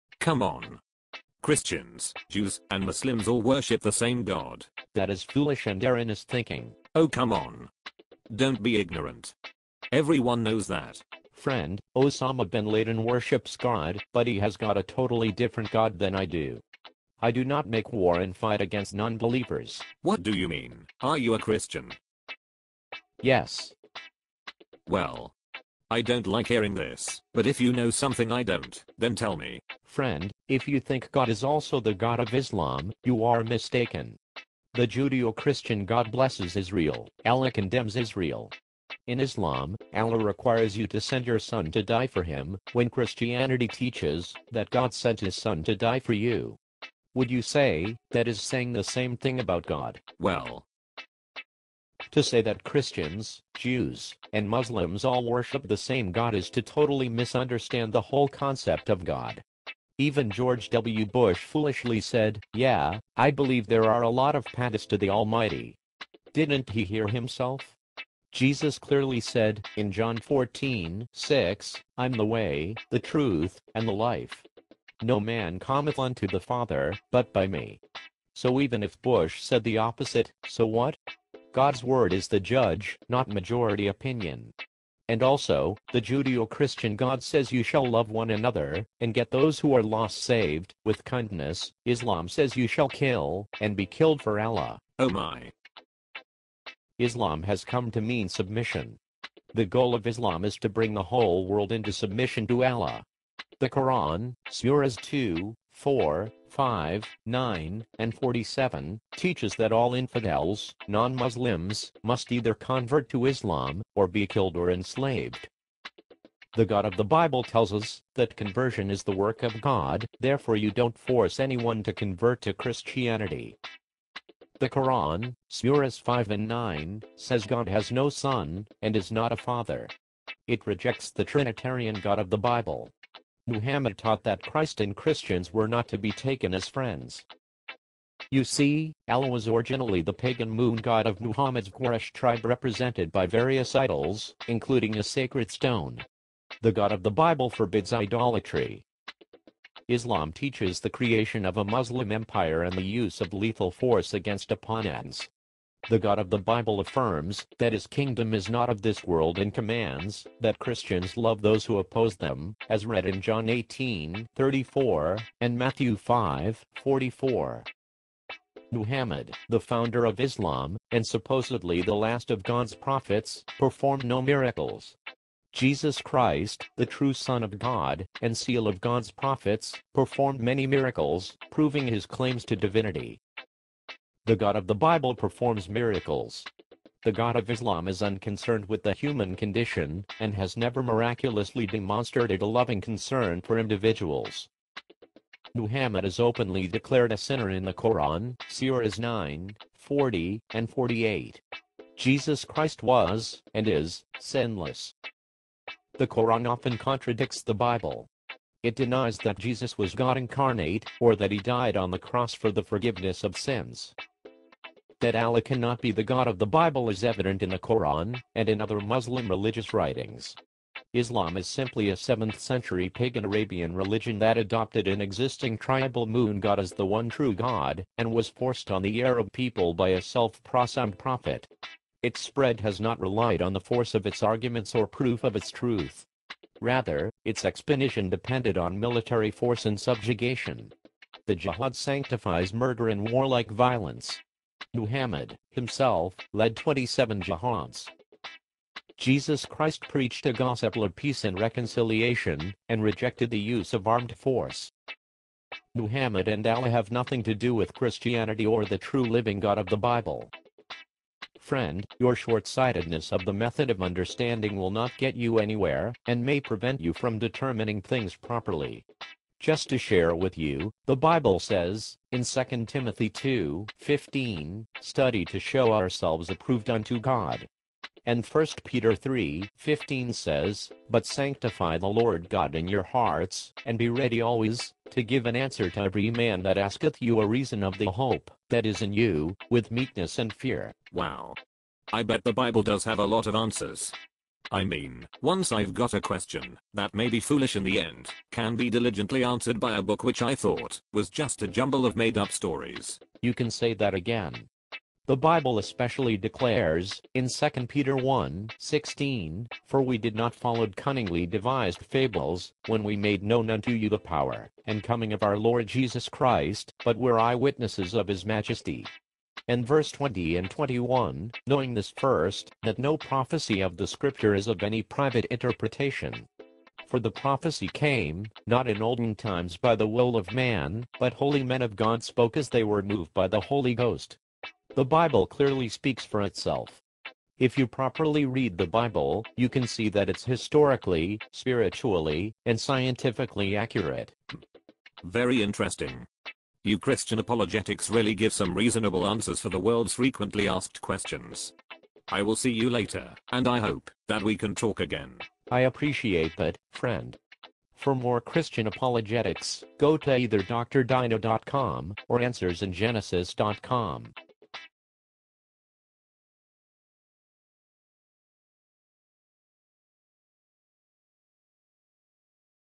Do Christians, Jews, & Muslims: Same God? NO!!! (xtranormal) (My First Video)
Why the Judeo-Christian God is not the same as the 'god' of Islam. Study & understand the facts before you go out, and try and determine things.